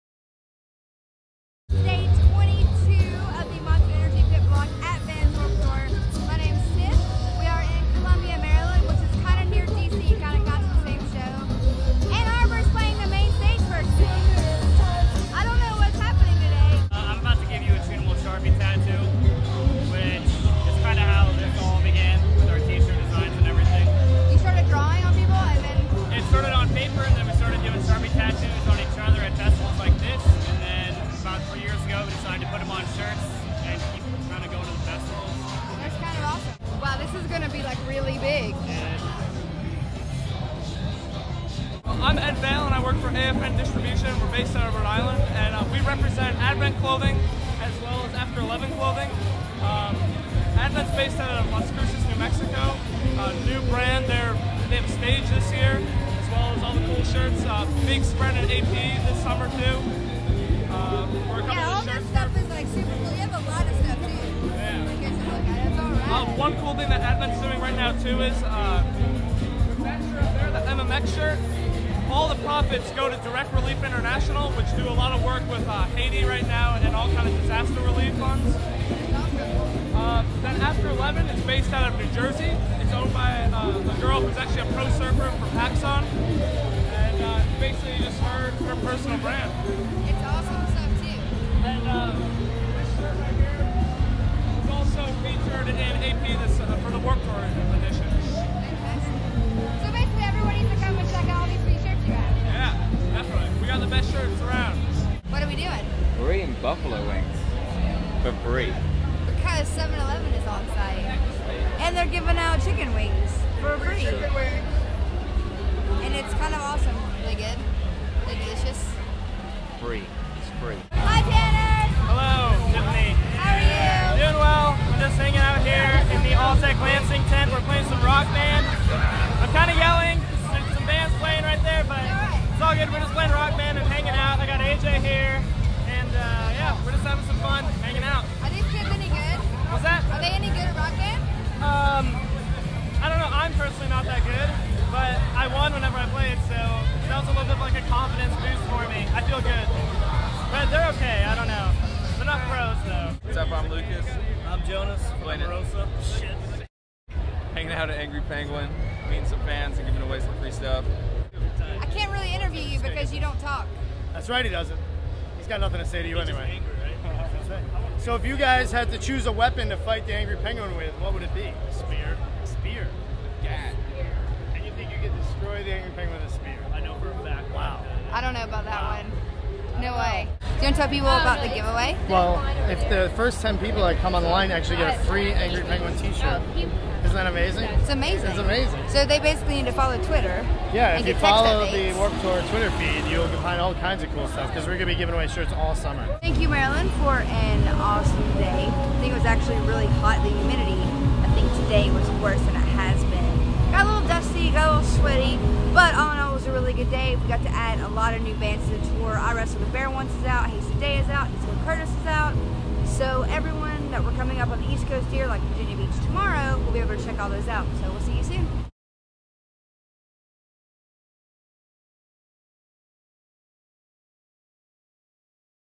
2010 Warped Pit Report: Day 22 - Columbia, MD
Angry Penguin and Emarosa, Disco Curtis, Advent Clothing, Choonimals and After Eleven Clothing.